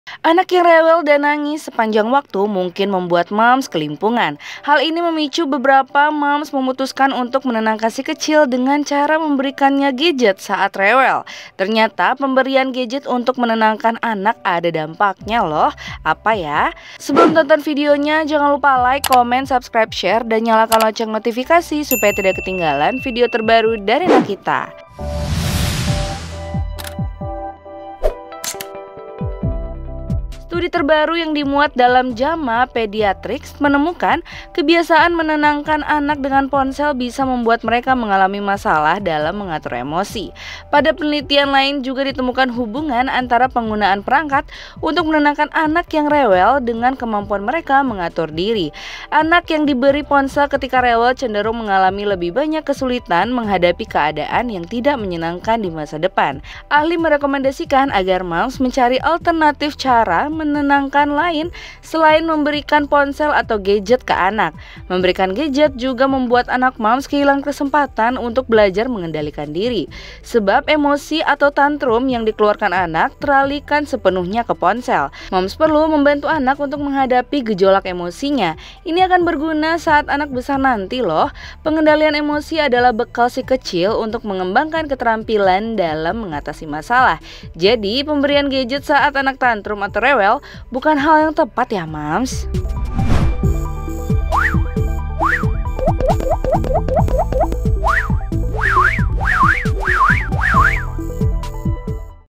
Menenangkan Anak Pakai Gadget, Efektif Sih Tapi Pertimbangkan Dampak Negatifnya Ini
Anak yang rewel dan nangis sepanjang waktu mungkin membuat Moms kelimpungan. Hal ini memicu beberapa Moms memutuskan untuk menenangkan si Kecil dengan cara memberikan gadget saat rewel. Ternyata, pemberian gadget untuk menenangkan anak ada dampaknya lho.

00:00 intro
00:30 Dampak Menenangkan Anak Pakai Gadget